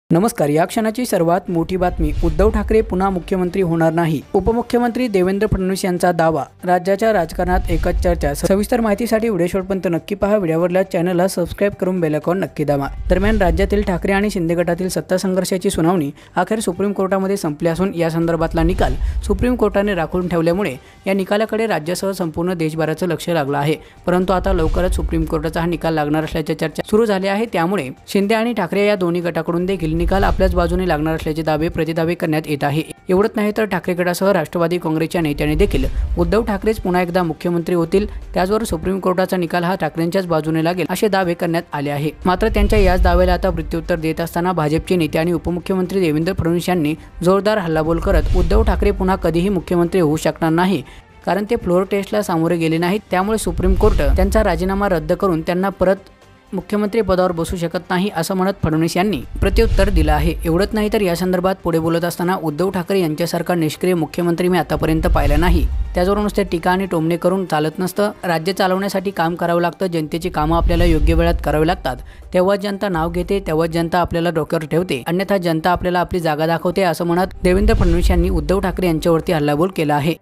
उद्धव ठाकरे पुन्हा मुख्यमंत्री होणार नाही फडणवीस संतापले राजकीय वर्तुळात चर्चा! latest Marathi News
उद्धव ठाकरे पुन्हा मुख्यमंत्री होणार नाही फडणवीस संतापले राजकीय वर्तुळात चर्चा! latest Marathi News Maharashtra